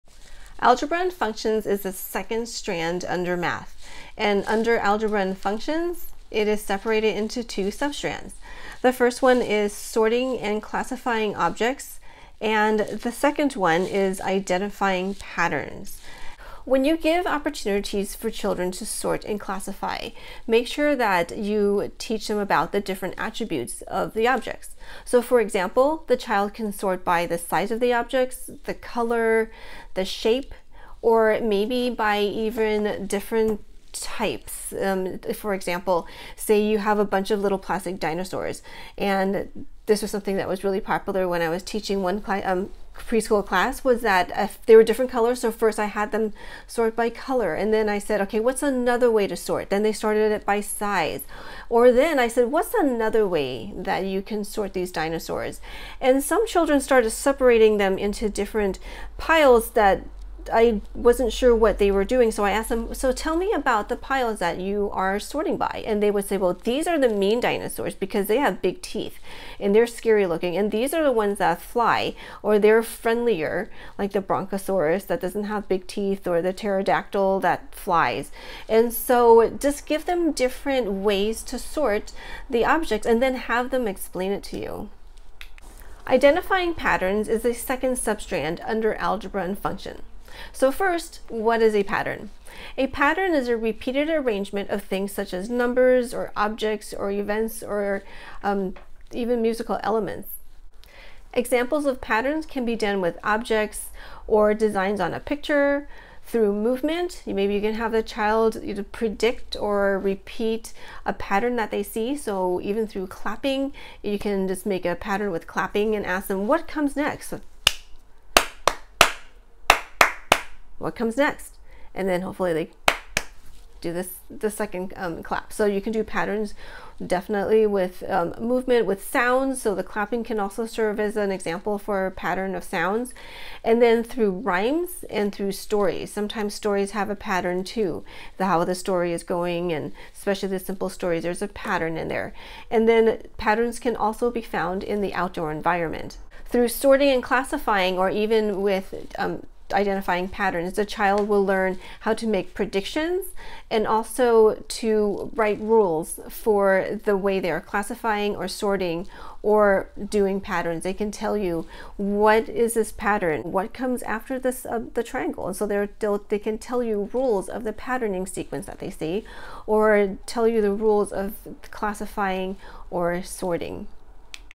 Algebra & functions In Early Childhood
Short description of Strand #2 of Math: Algebra and functions in early childhood education